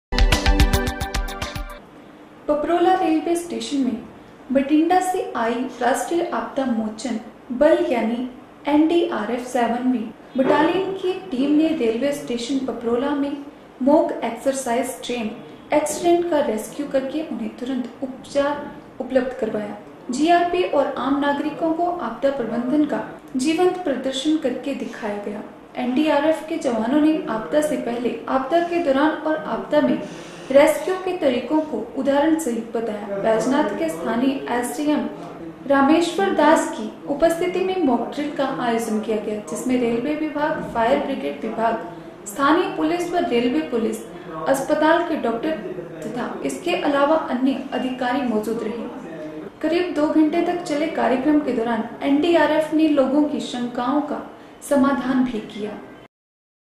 NDRF in railway station Paprola told how to deal with the disaster
पपरोला  रेलवे स्टेशन  में भटिंडा  से आई राष्ट्रीय आपदा मोचन बल यानि एनडीआरएफ 7 वीं बटालियन की टीम ने रेलवे स्टेशन पपरोला  मैं मोक एक्सरसाइज ट्रेन एक्सीडेंट का रेस्क्यू   करके उन्हें तुरंत उपचार उपलब्ध करवाया जीआरपी और आम नागरिकों को आपदा प्रबंधन का जीवंत प्रदर्शन करके दिखाया गया